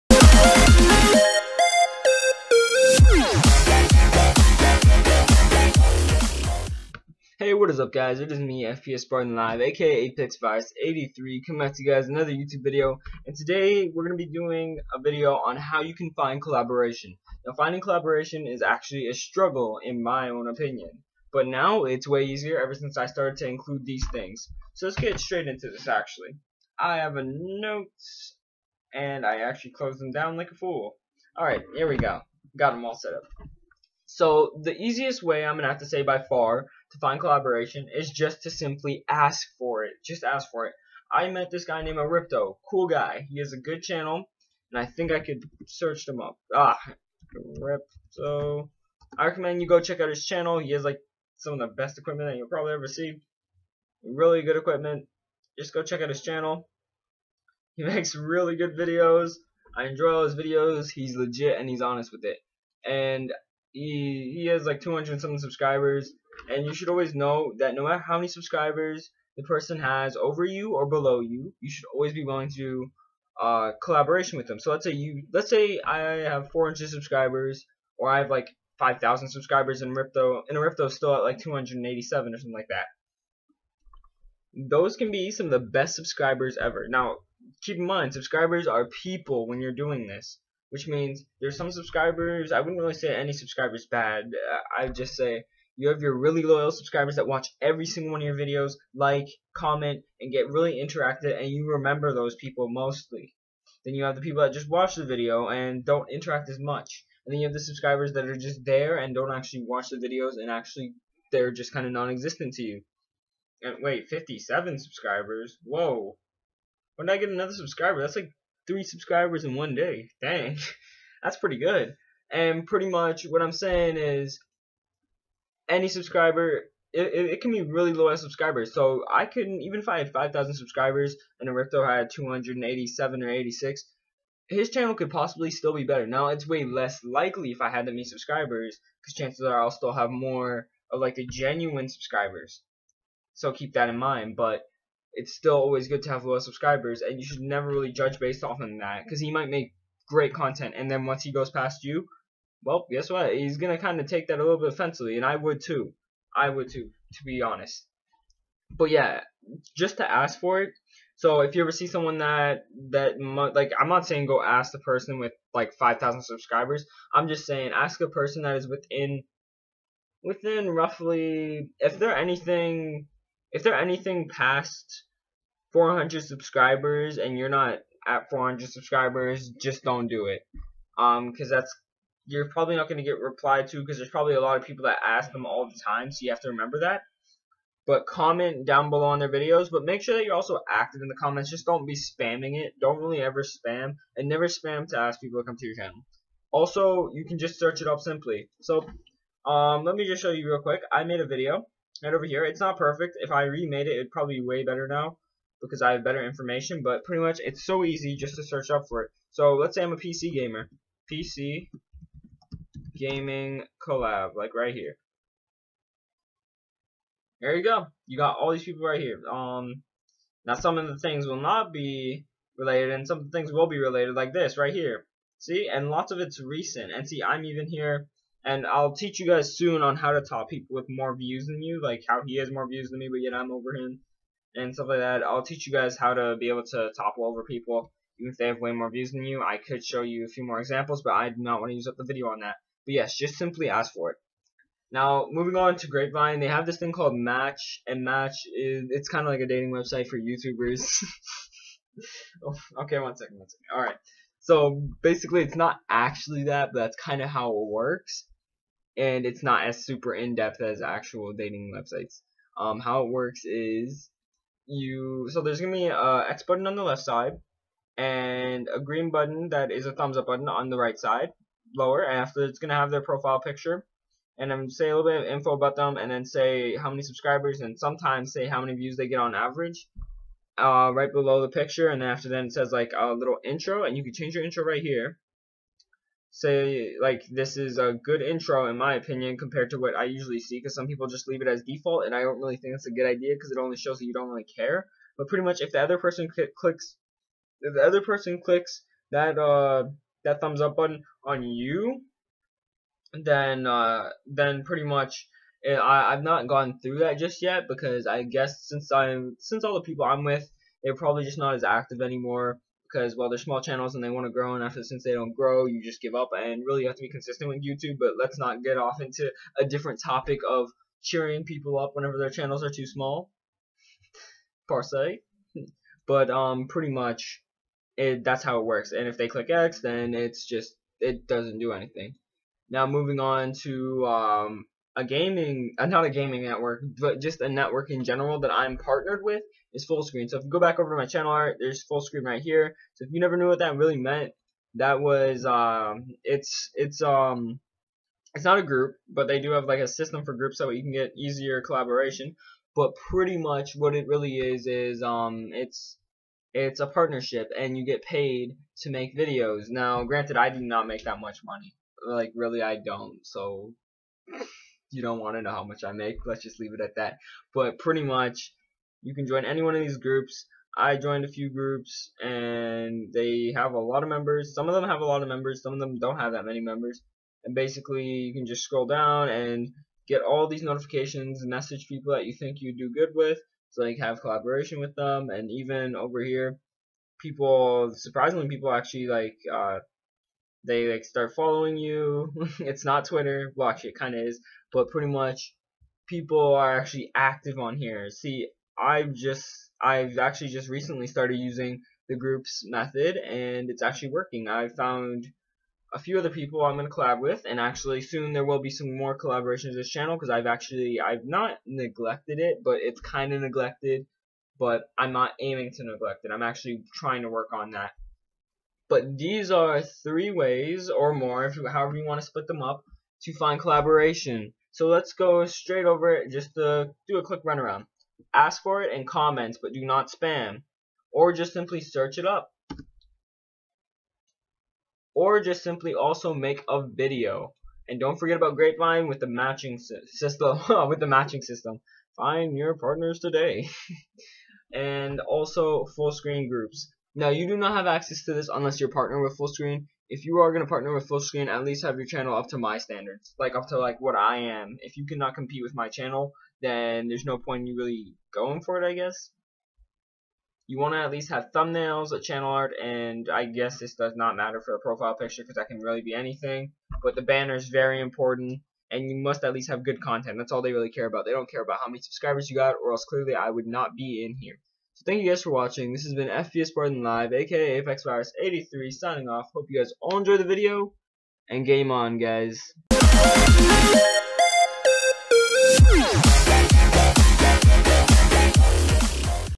How To Find Collaboration - Youtube
Her in this quick video I'll be showing some ways of how you can find other YouTubers to collaborate with.